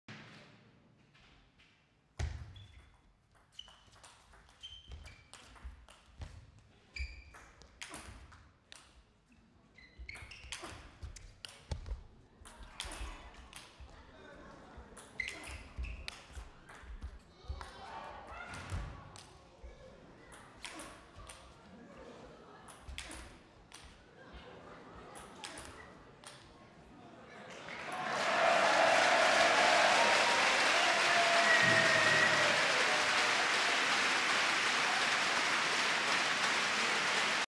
23.10.2011 Ballwechsel des Tages
Borussia Düsseldorf gegen TTC matec Frickenhausen

Dieser tolle Ballwechsel stammt aus der Partie Janos Jakab gegen Torben Wosik, den Wosik für sich entscheiden konnte. Das Match ging nach 4 Sätzen allerdings an den Ungar.

Saison 2011/12